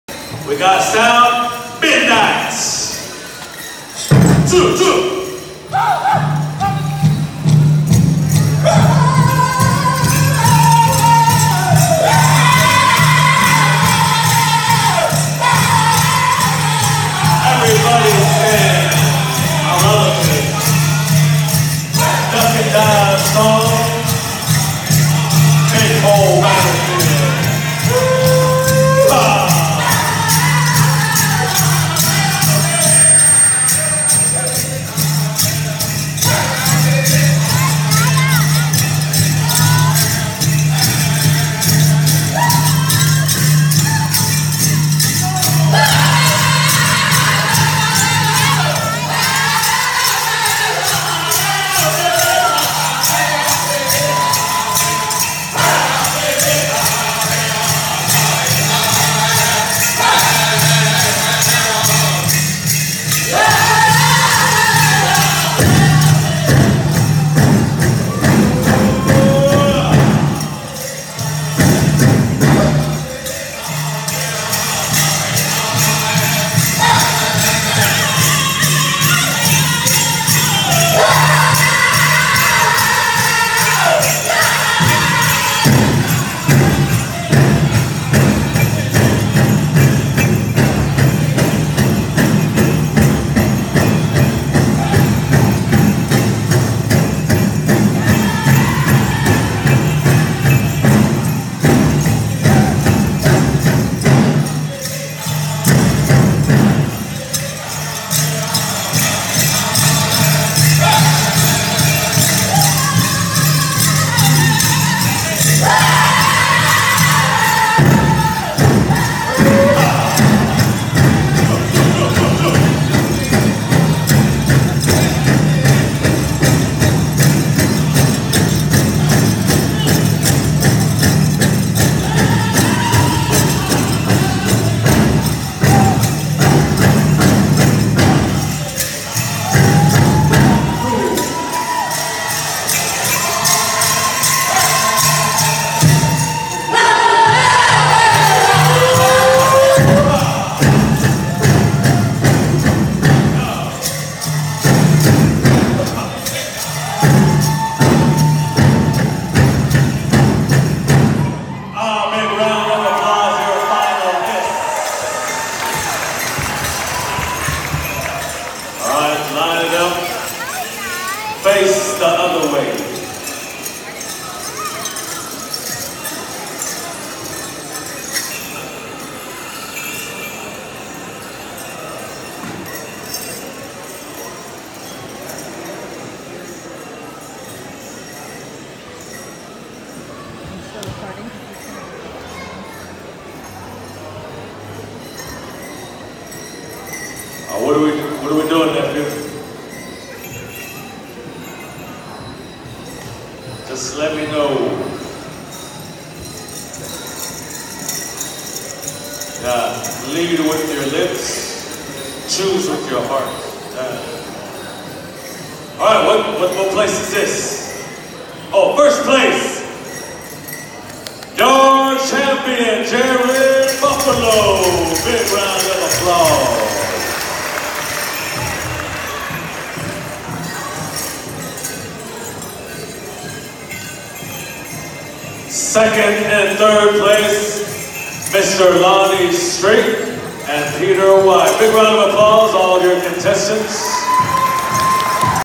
Mens Traditional special Tulalip2019
Headman danced Tommy Lee Whiteplume special too 3 duck and dive